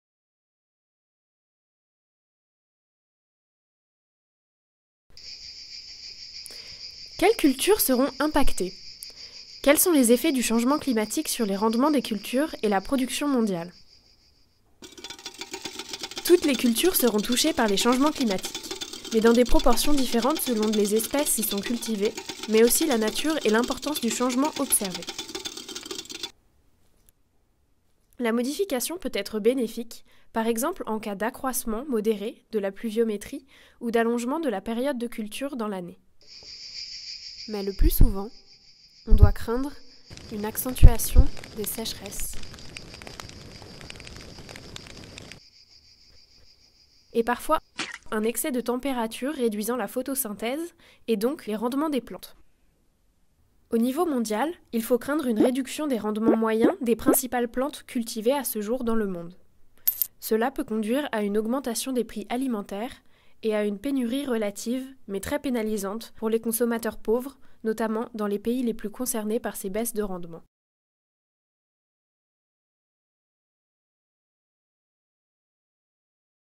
AGRICLIM — Question 2/10
AGRICLIM, ce sont les réponses d'un(e) spécialiste agriculture-climat-forêt de l'AAF aux questions actuelles, mises en image par de jeunes créatifs de l'école Estienne.
10 films, 1 film par jour, mis en ligne en amont de la COP21.

Question 2
Quelles cultures seront impactées ?
Quels sont les effets du changement climatique sur les rendements des cultures et la production mondiale ?

Film AGRICLIM — Question 2 : auteur, André Neveu ; réalisation : Lola Pradeilles, Clara Morineau, Hannah Turpaud, Blanche Garnier.
Une co-production Académie d'Agriculture de France, Ecole Estienne, Institut Pierre Simon Laplace des sciences de l'environnement.

Sur une idée d'Agnès Ricroch.
Collaboration de Katia Laval.
Direction pédagogique : Eric Boisseau, Patrick Pleutin, Emmanuelle Tieu (Design Graphique médias numériques).